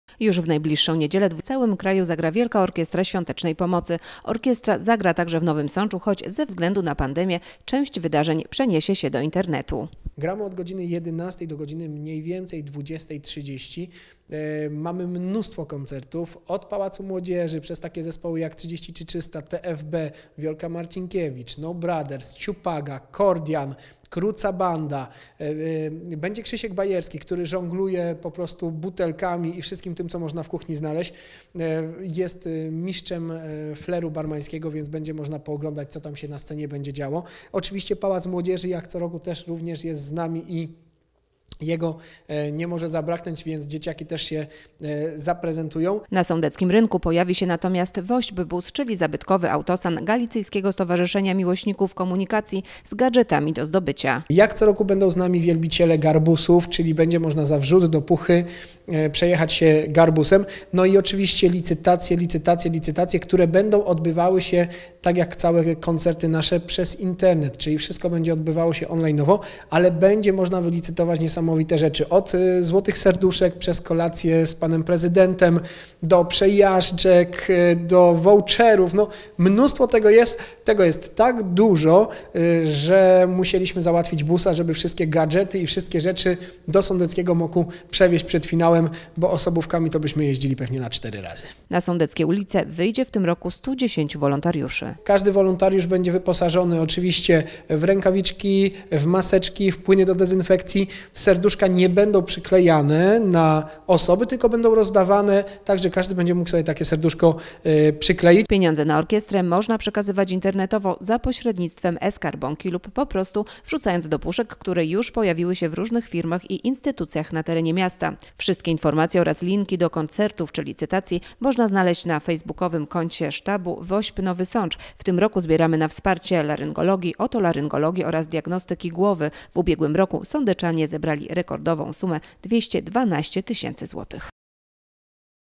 W Nowym Sączu zagra WOŚP
Już w niedzielę, 31 stycznia w całym kraju zagra WOŚP. Orkiestra zagra także w Nowym Sączu, choć ze względu na pandemię część wydarzeń przeniesie się do internetu. W sieci będzie można na żywo oglądać koncerty odbywające się w MOK oraz licytować wystawione na aukcję przedmioty.

W tym roku na sądeckich ulicach pojawi się 110 wolontariuszy. Na rynku pojawi się także WOŚPBUS z gadżetami a za datek do puszki będzie się można przejechać garbusem.

Wszystkie informacje oraz linki do transmsisji i licytacji na facebookowym profilu: WOŚP Nowy Sącz.

W tym roku zbieramy na wsparcie laryngologii, otolaryngologii oraz diagnostyki głowy.

W ubiegłym roku Sądeczanie zebrali rekordową sumę 212 tys zł.